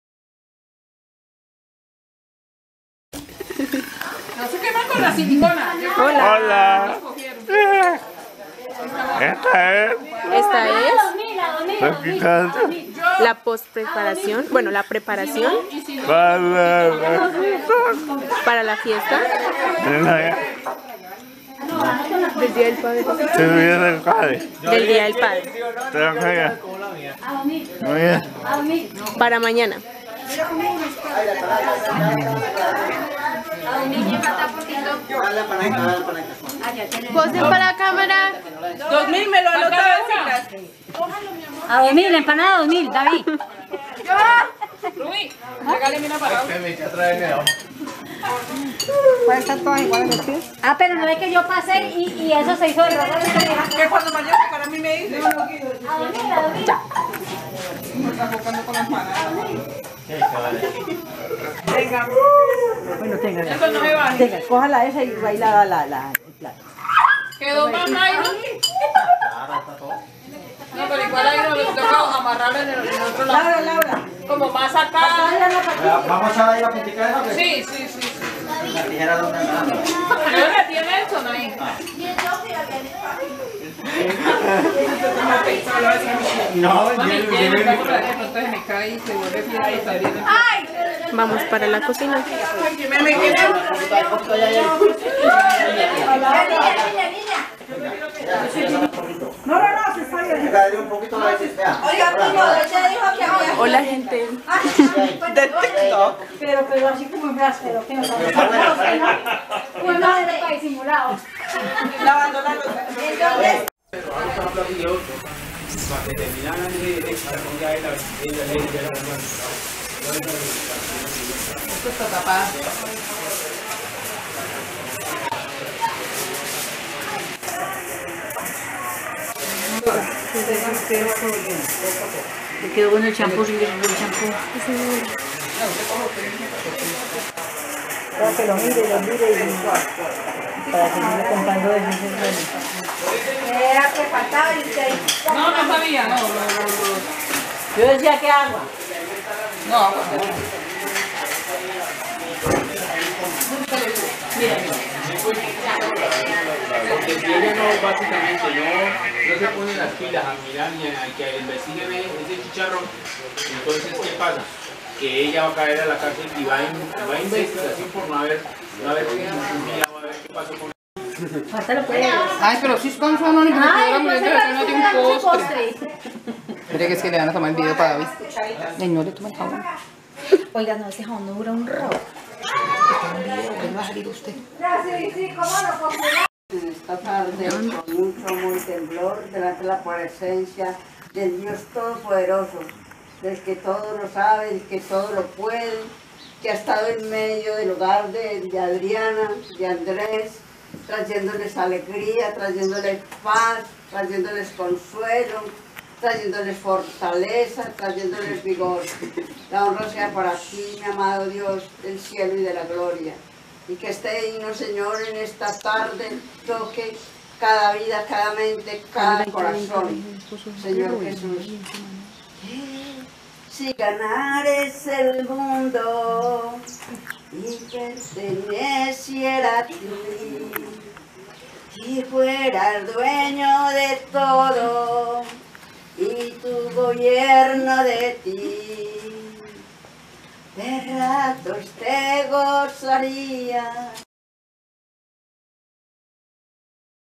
Celebración de el dia de el padre parte 1!!
ASI FUE LA PREPARACION PARA EL EVENTO DE EL DIA DE EL PADRE!!